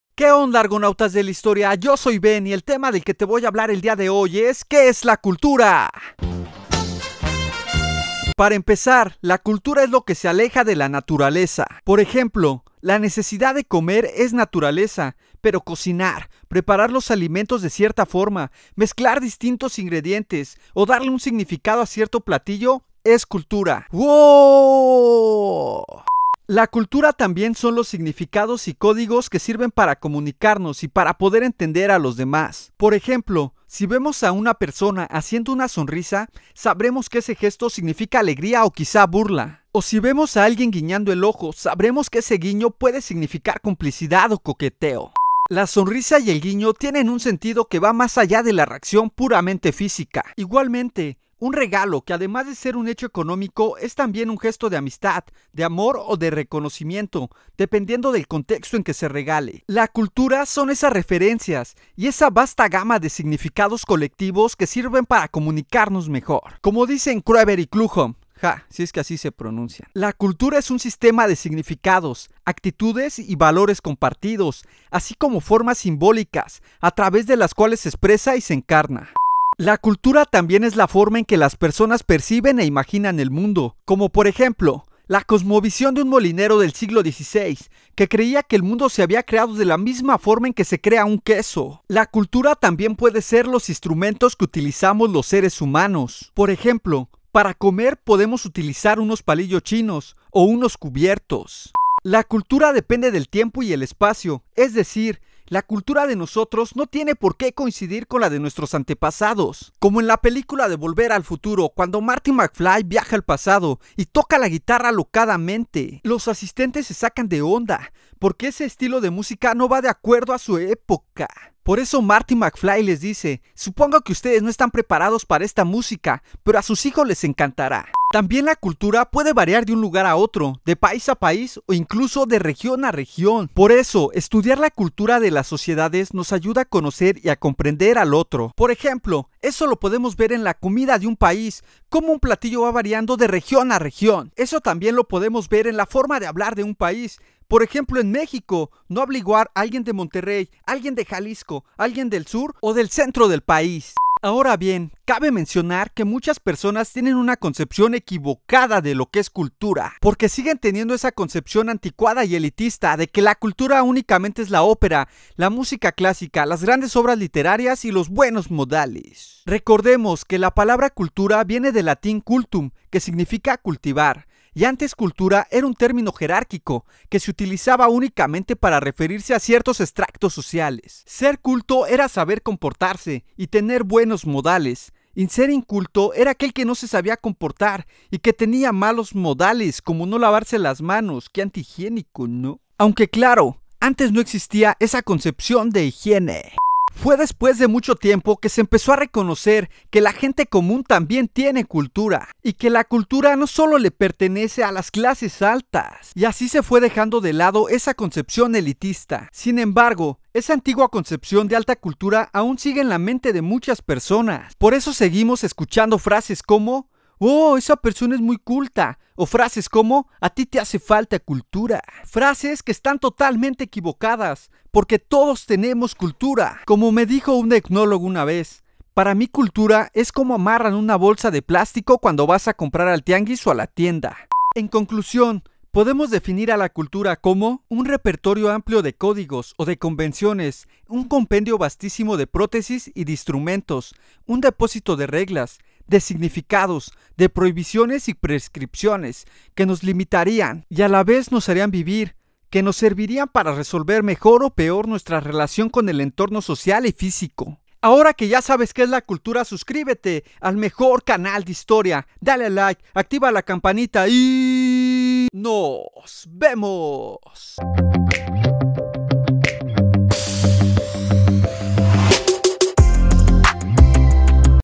🚀 ¿QUÉ ES la CULTURA? (definición más ejemplos)
APRENDE que es CULTURA con ejemplos Súper fáciles. En MENOS DE 5 MINUTOS descubrirás TODO LO QUE NECESITAS SABER sobre Cultura (su definición, su etimología, sus características y bla, bla,bla). Créeme te vas a sorprender.  Después de este video tu concepción sobre Cultura CAMBIARA.
DALE AL LIKE y
"Muchas personas siguen teniendo una anticuada concepción de Cultura, ya es hora de que esto termine..."
Ben Romero (Historiador)